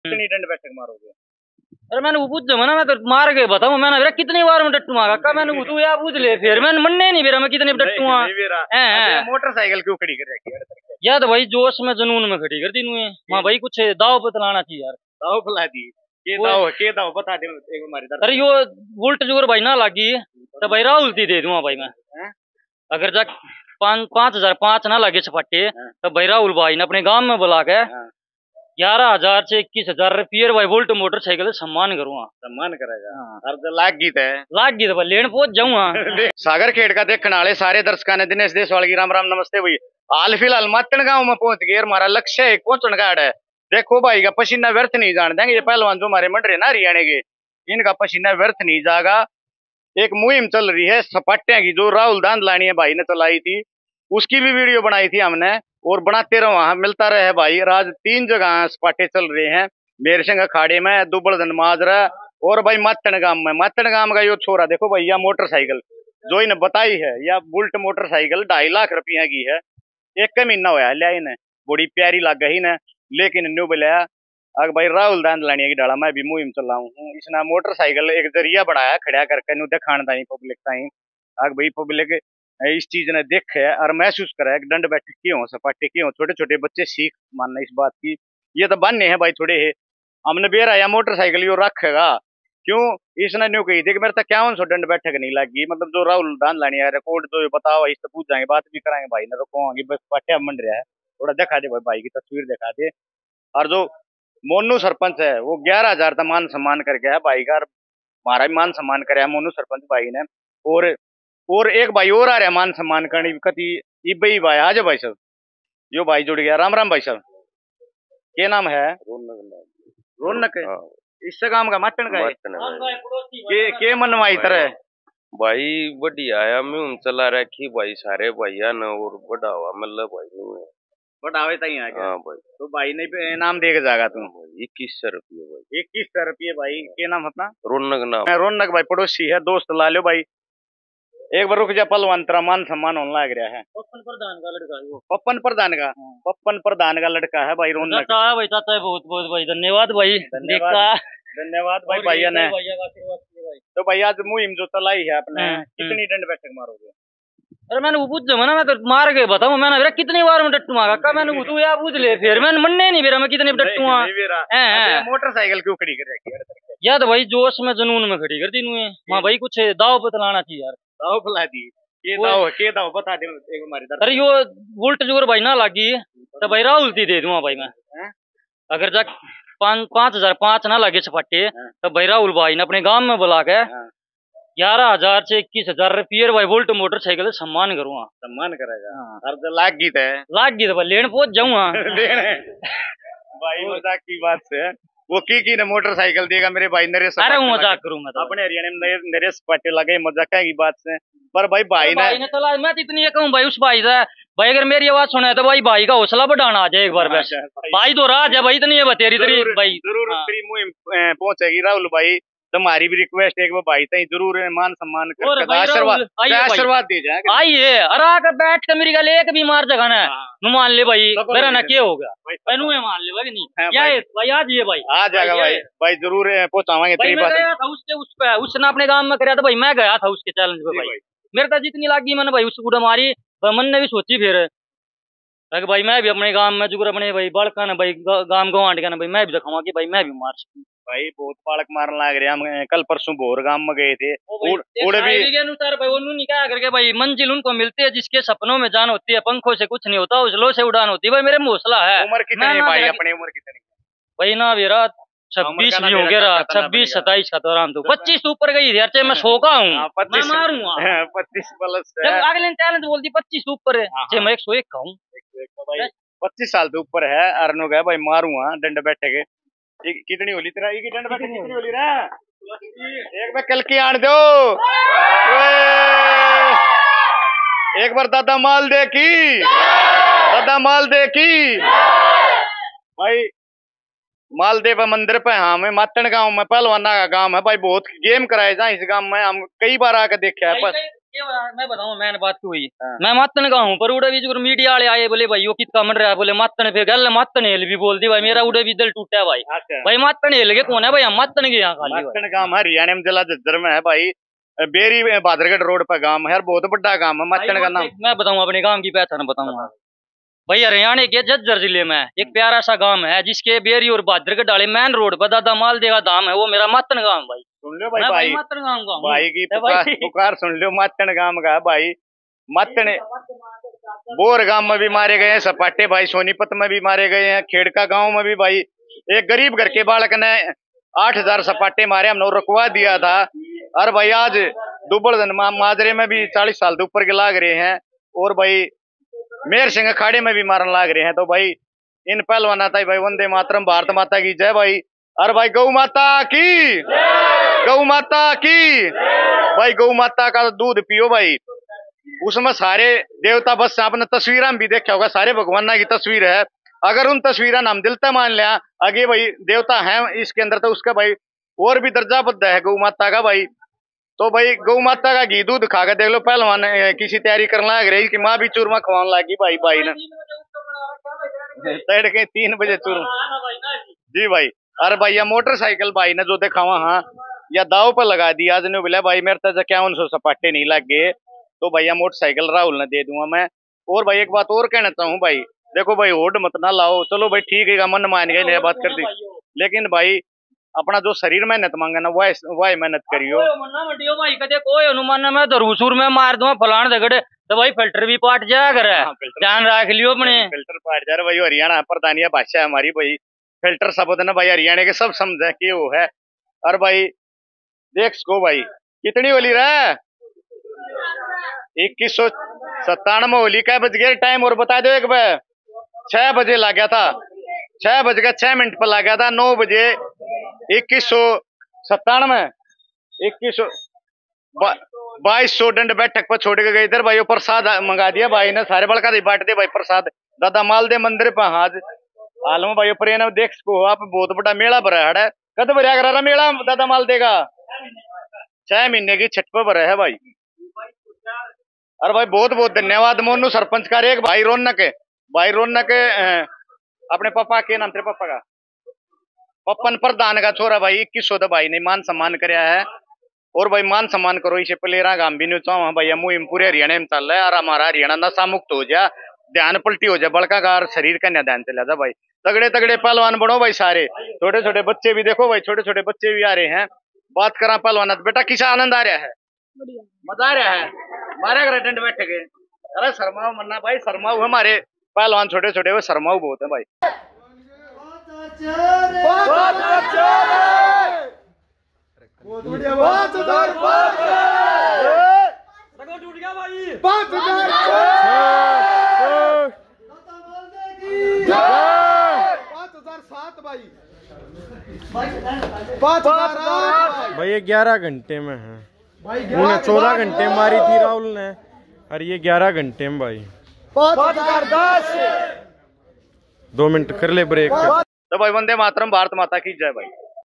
दाव पर लगा दी बुले"ट मोटरसाइकिल? राहुल Dhandlaniya , को किया याद, मातन गांव का पहलवान।
गांव पर लगा दी बुले"ट मोटरसाइकिल? राहुल Dhandlaniya , को किया याद, मातन गांव का पहलवान।